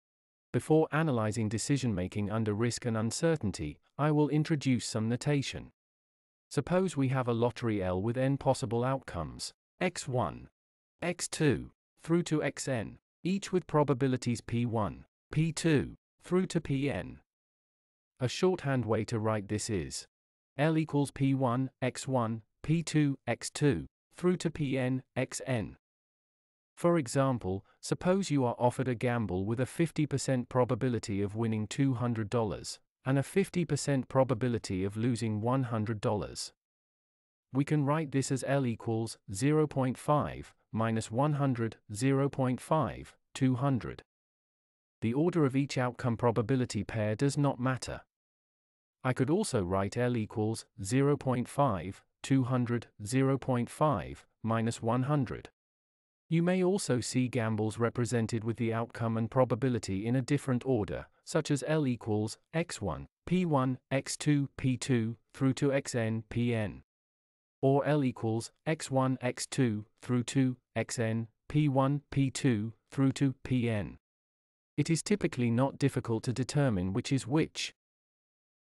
Notation
This video introduces notation used in the Decision making under risk and uncertainty playlist in Notes on Behavioural Economics.